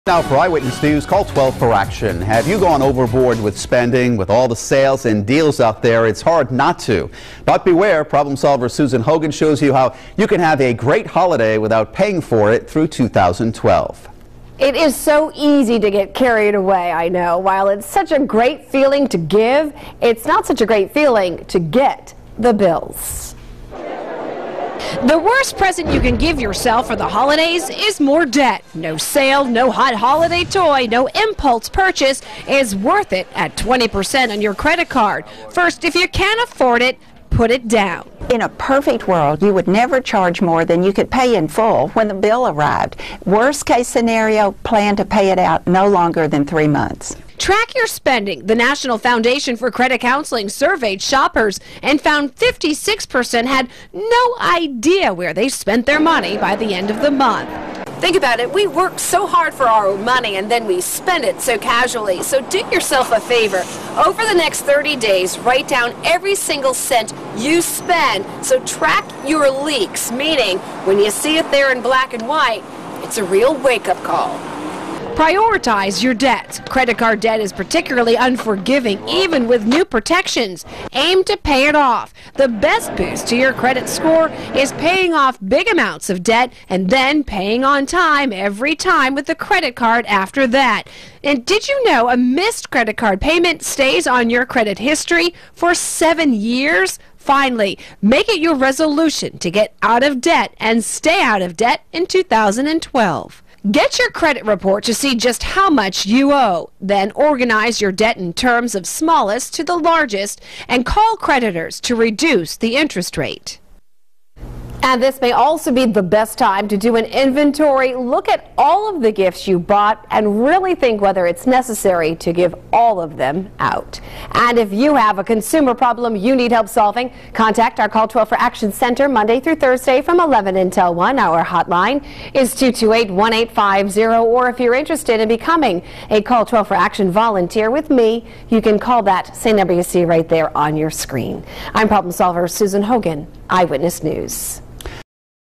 CFA: Avoiding Holiday Debt
The temptation to spend big during the holiday season is strong, but usually results in bigger bills come January.